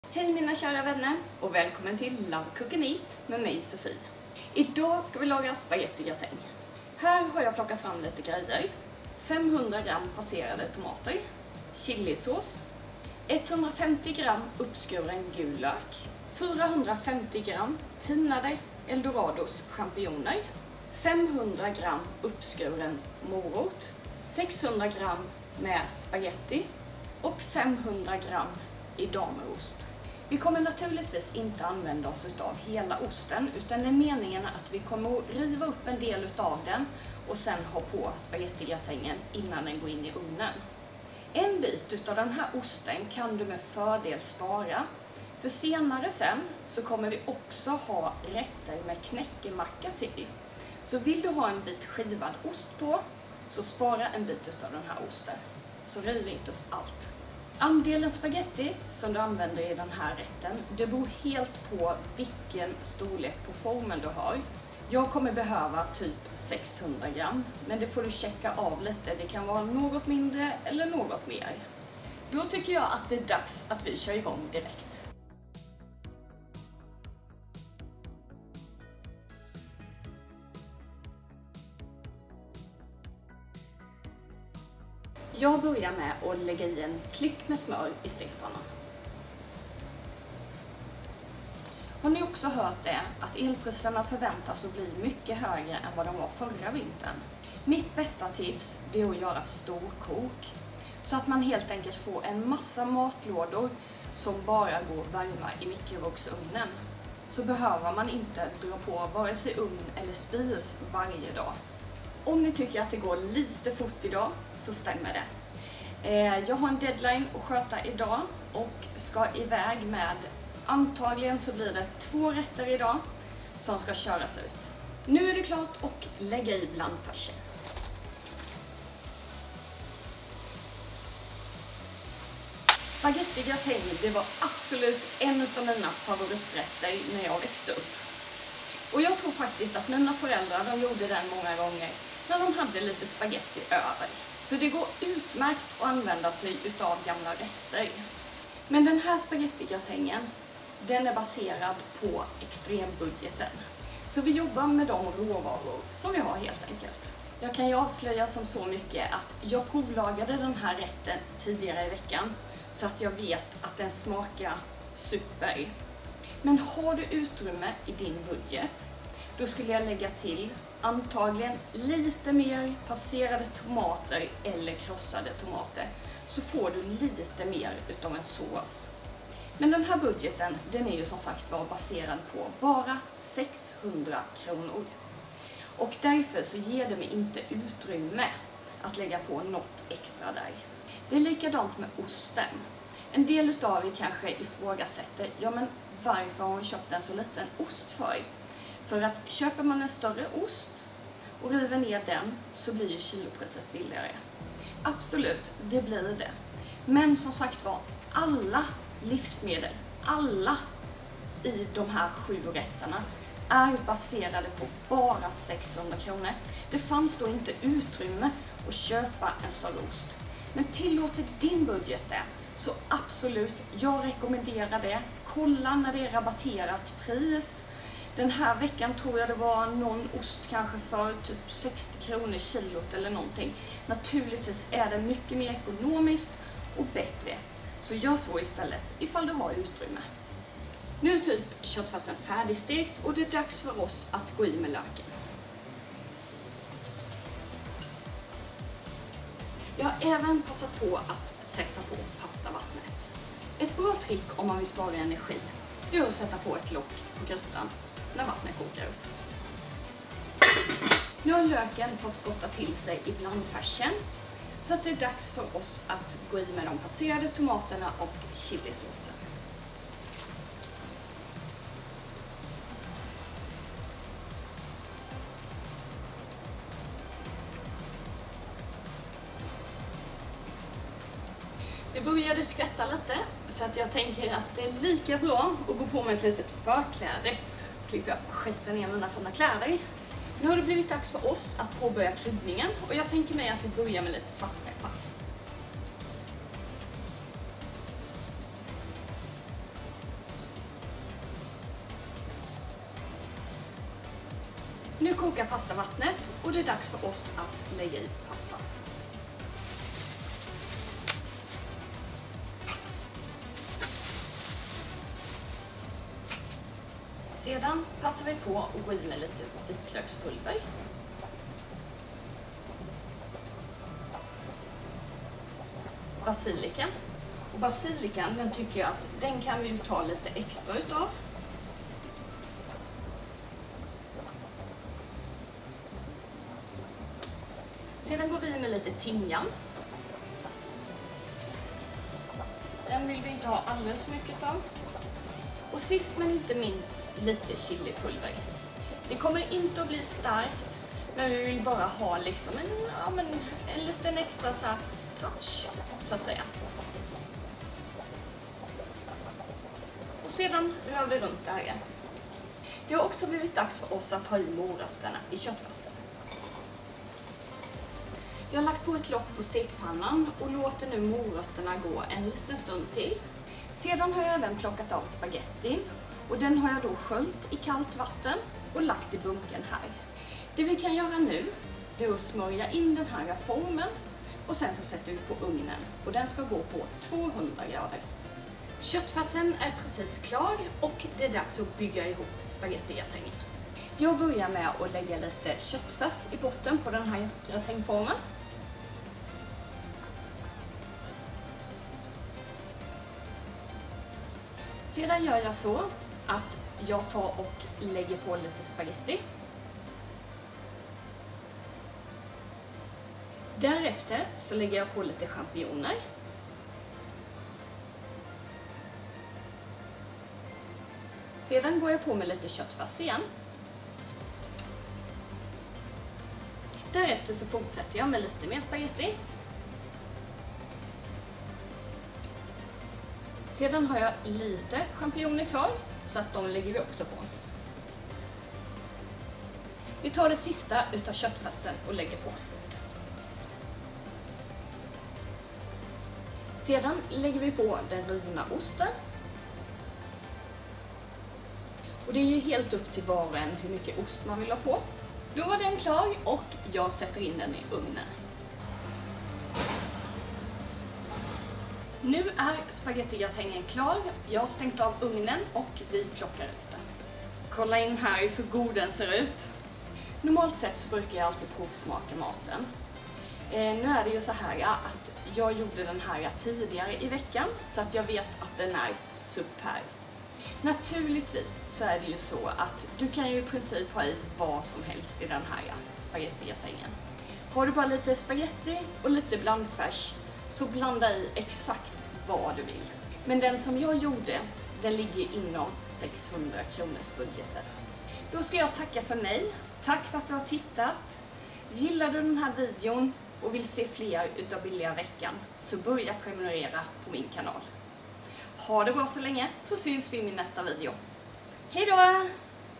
600 kr - 4 personer - 1 vecka - GÅR DET? (3/7)
I den tredje delen av "Billiga Veckan - Extrembudget" lagar jag min barndomsfavorit, spaghettigratäng med köttfärs.
Alla ingredienser med pris finns listat i videon.

"Billiga Veckan - Extrembudget" vänder sig till dig som vill/behöver klara dig på en extremt låg matbudget, för dig som är student eller för dig som vill ha ny inspiration.